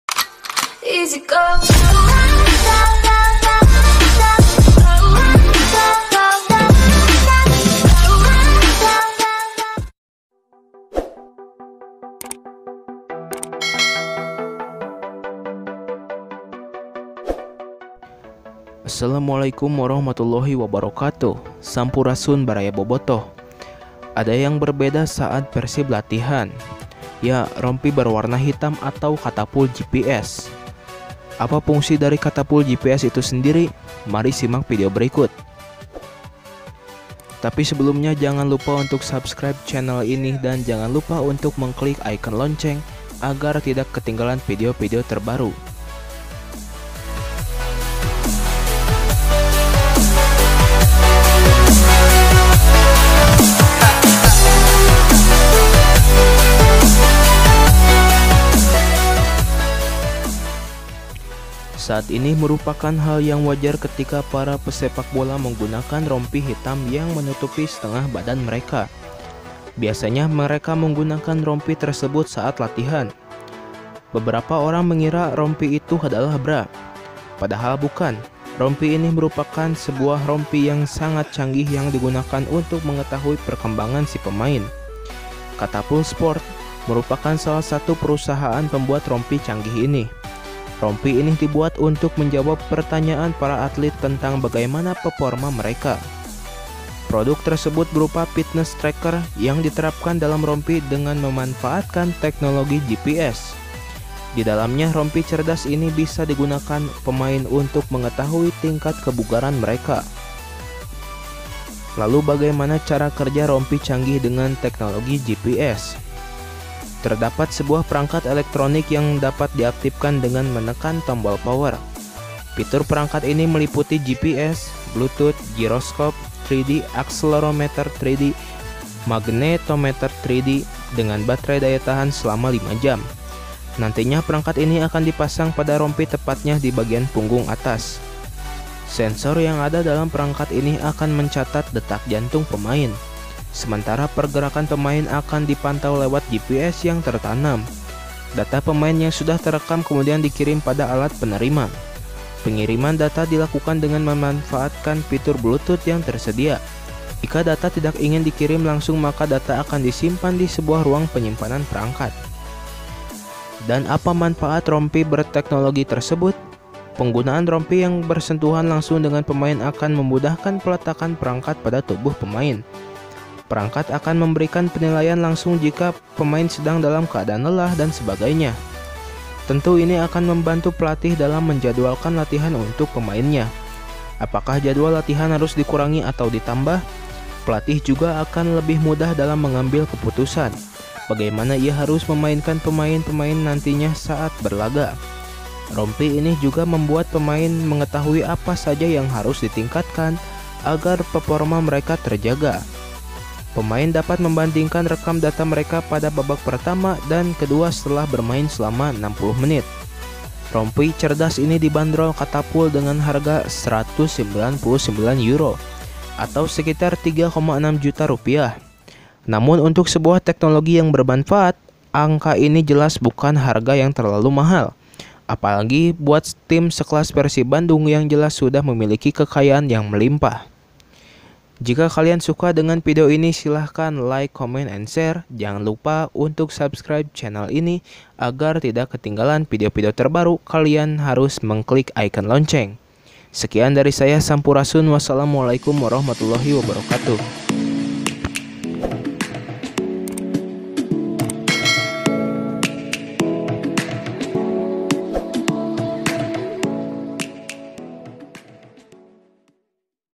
Fungsi rompi canggih di latihan persib
📢Sampurasun baraya bobotoh

Latihan persib menggunakan rompi canggih catapult gps.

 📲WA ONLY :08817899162
🌏Bandung,westjava,Indonesia